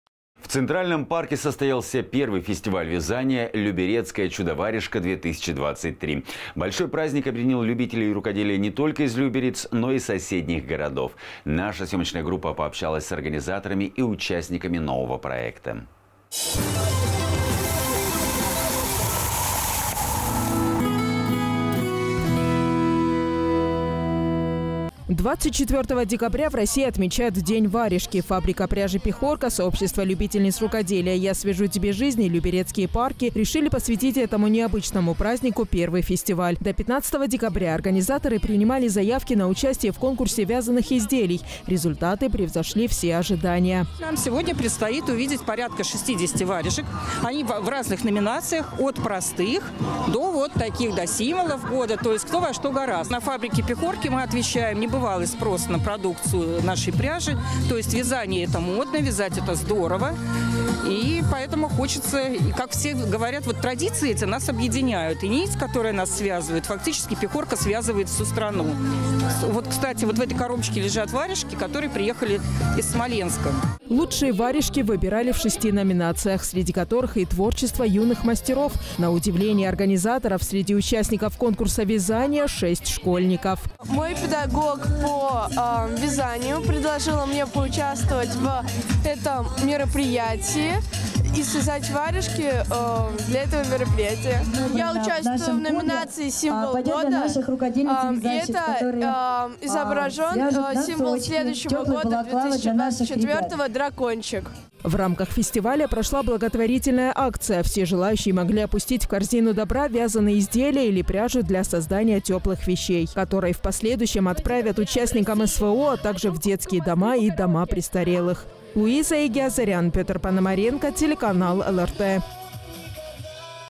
«Люберецкая чудо варежка 2023»
В Центральном парке состоялся первый фестиваль вязания «Люберецкая чудо-варежка 2023». Большой праздник объединил любителей рукоделия не только из Люберец, но и соседних городов. Наша съемочная группа пообщалась с организаторами и участниками нового проекта.

24 декабря в России отмечают день варежки. Фабрика пряжи «Пехорка», сообщество любительниц рукоделия «Я свяжу тебе жизнь» и Люберецкие парки решили посвятить этому необычному празднику первый фестиваль. До 15 декабря организаторы принимали заявки на участие в конкурсе вязаных изделий. Результаты превзошли все ожидания.

Ирина Рогозина, пресс-секретарь фабрики «Пехорский Текстиль»: «Нам сегодня предстоит увидеть порядка 60 варежек, они в разных номинациях. От простых до вот таких символов Нового года, то есть, кто во что горазд. На фабрике «Пехорка» мы отмечаем небывалый спрос на продукцию нашей пряжи, то есть вязание – это модно, вязать – это здорово и традиции эти нас объединяют и фактически «Пехорка» связывает всю страну. Вот, кстати, в этой коробочке лежат варежки, которые приехали из Смоленска».

Лучшие варежки выбирали в 6 номинациях, среди которых и «Творчество юных мастеров». На удивление организаторов, среди участников конкурса вязания – 6 школьников.

Соня Заикина, участница фестиваля «Люберецкая чудо-варежка 2023»: «Мой педагог по вязанию предложила мне поучаствовать в этом мероприятии и связать варежки для этого мероприятия. Я участвую в номинации «Символ года». Это изображен символ следующего года 2024 – дракончик».

В рамках фестиваля прошла благотворительная акция. Все желающие могли опустить в корзину добра вязаные изделия или пряжу для создания теплых вещей, которые в последующем отправят участникам СВО, а также в детские дома и дома престарелых.